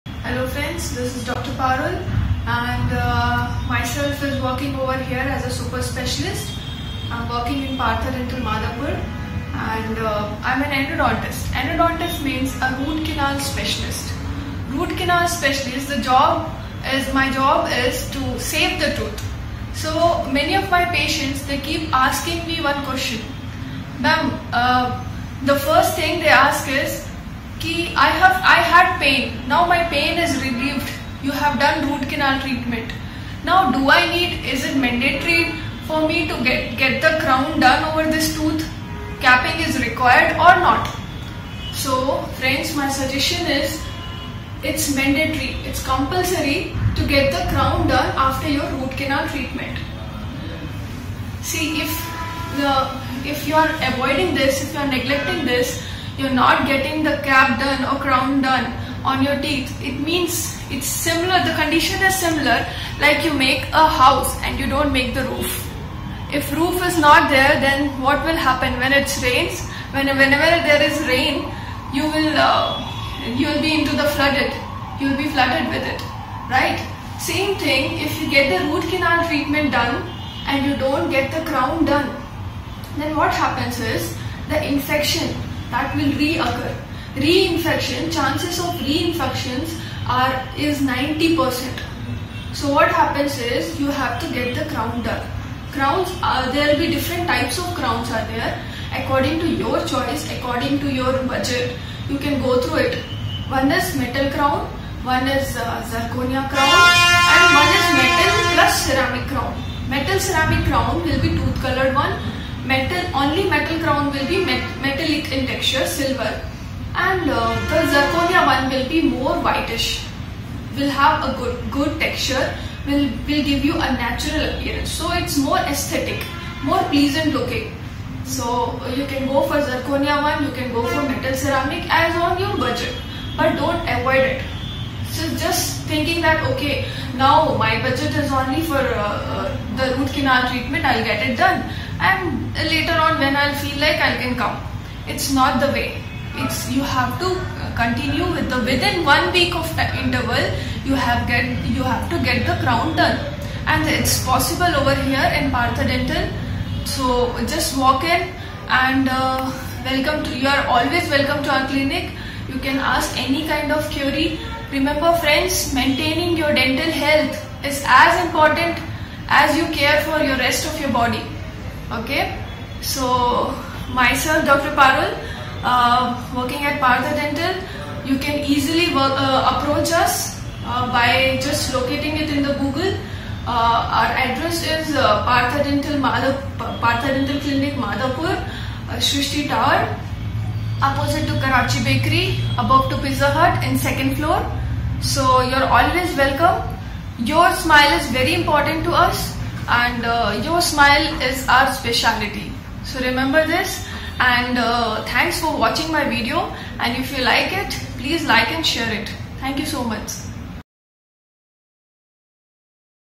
Why neglecting your teeth can have serious consequences? II Partha Dental II
As should be obvious, tooth rot begins off moderate however can rapidly quicken once it infiltrates past the finish layer of the tooth. Once in a while you might not have any indications of tooth rot whatsoever until the point when it gets into the last stage. Therefore you should visit a dental specialist immediately in the event that you see any manifestations of tooth rot so you can counteract further, more risky complexities.